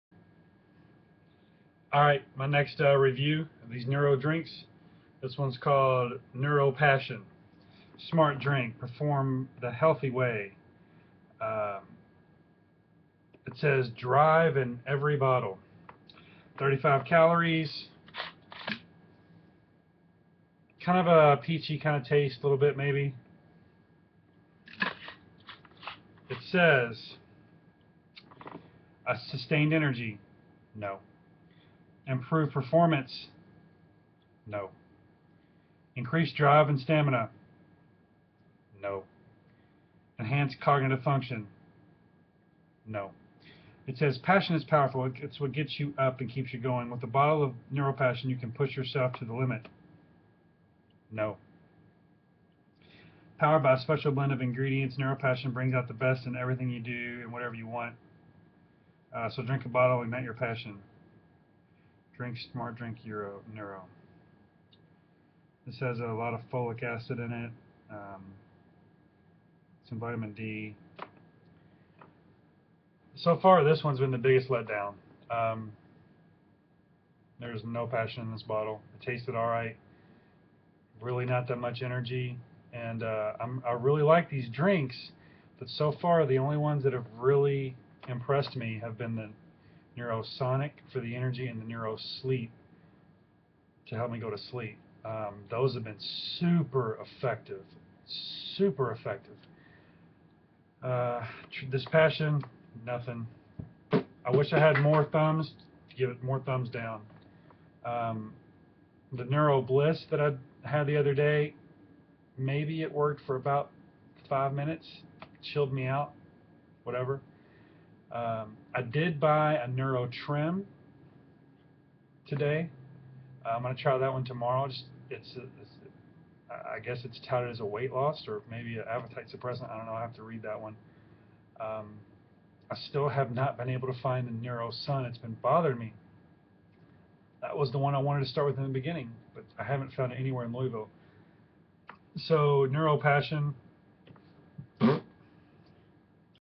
Neuro Passion Review
I love the "Neuro" drinks, but so far, only Neuro Sonic and Neuro Sleep have really done anything for me. This drink was the worst of the bunch so far. Zero Passion, almost no energy.. but hey. a 14 oz drink that tasted alright and only had 35 calories so it wasn't all for nothing lol